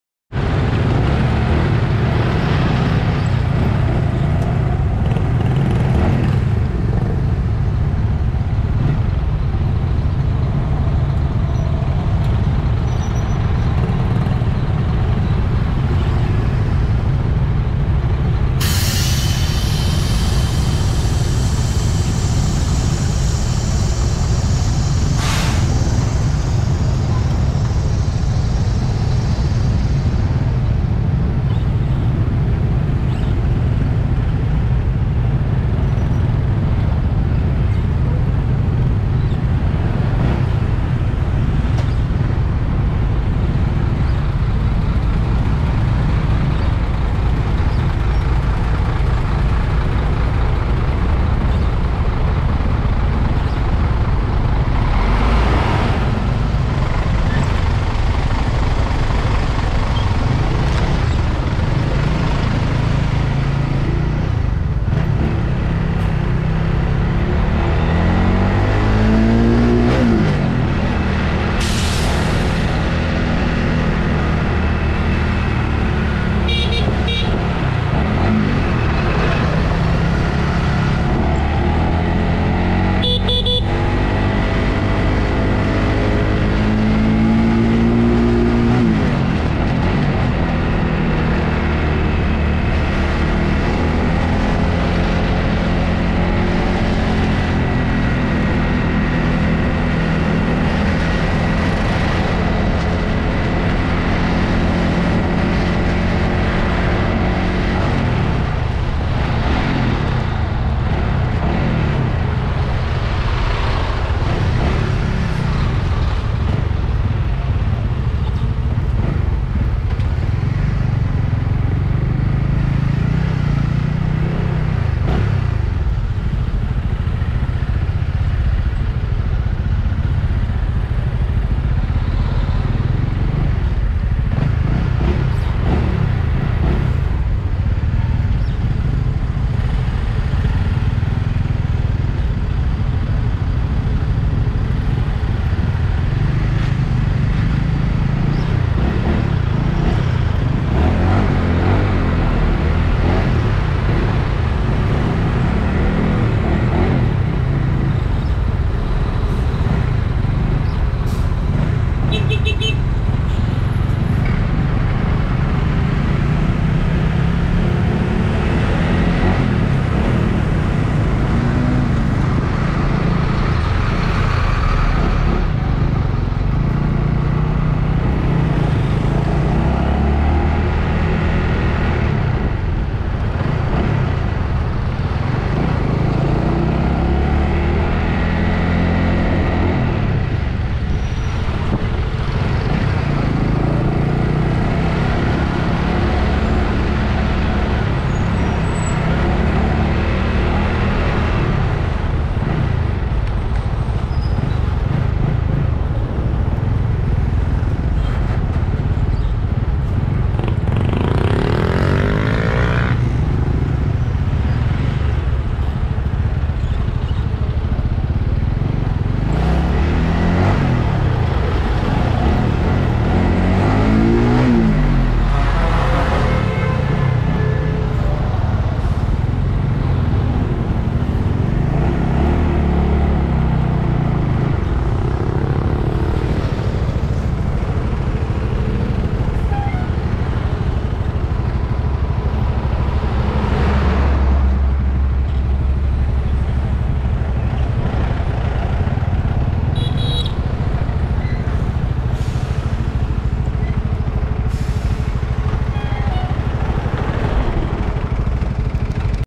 Tai Nạn - Kẹt Xe - Chen Lấn Kinh Hồn - P²
Lúc 8h Ngày 12 tháng 5 năm 2021
Đoạn QL1a Hướng TX Cai Lậy về Bình Phú